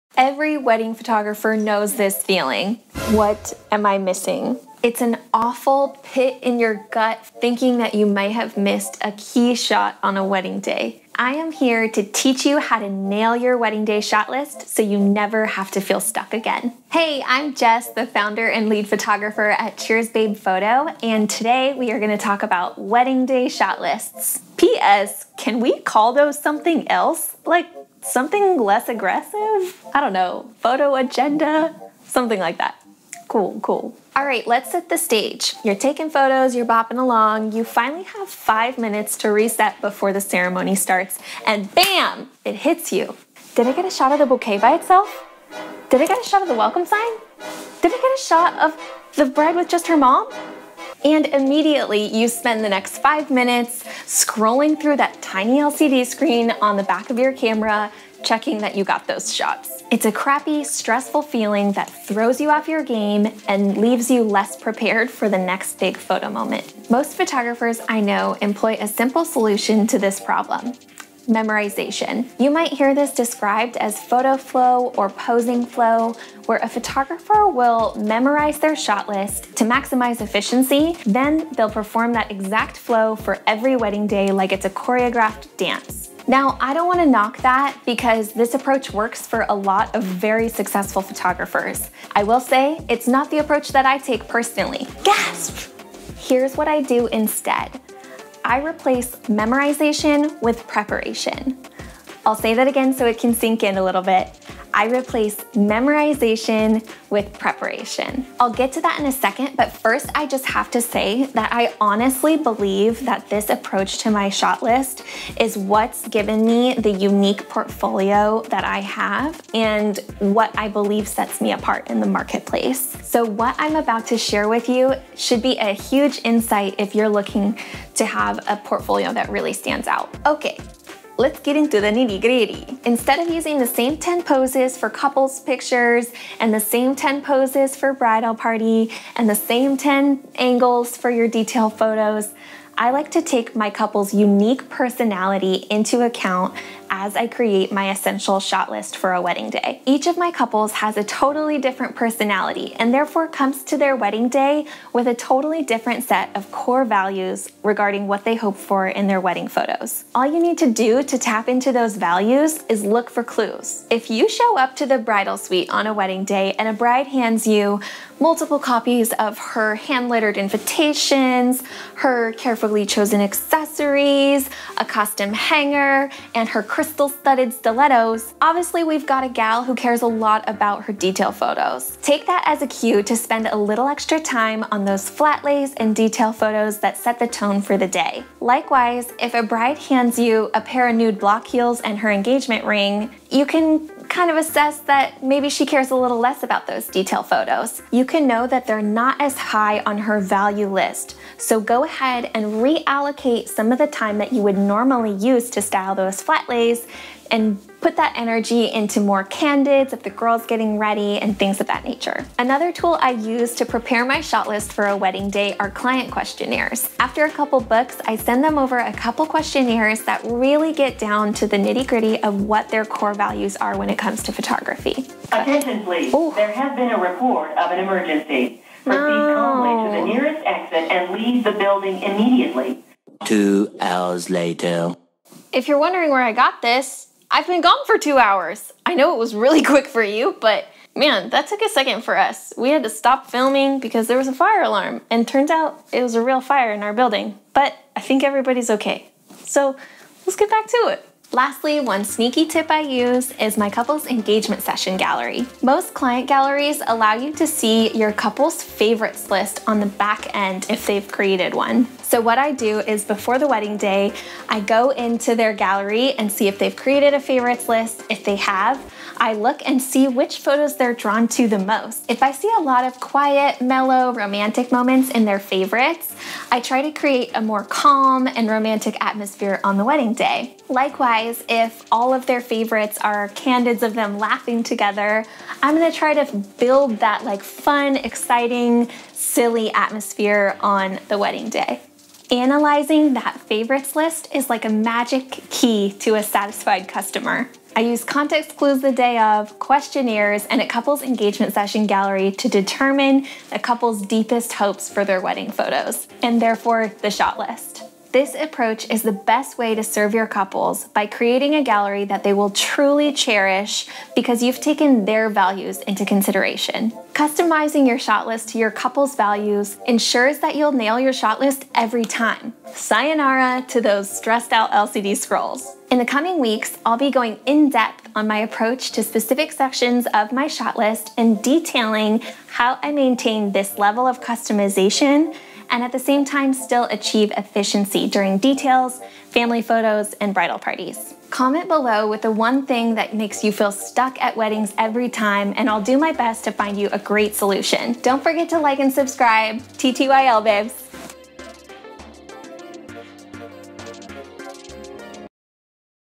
Wedding Day Shot List 📸 How to Never Feel Stuck Again
Every wedding photographer knows this feeling: WHAT am I missing? It's an awful, pit-in-your-gut, wondering if you missed a key photo on a wedding day.

I am here to teach you how to nail your wedding day shot list so you never have to feel stuck again. You'll learn my biggest tip when it comes to approaching a wedding day shot list. So whether you're a beginner or professional, I hope that this helps!

Let's be Insta friends: @cheersbabepresets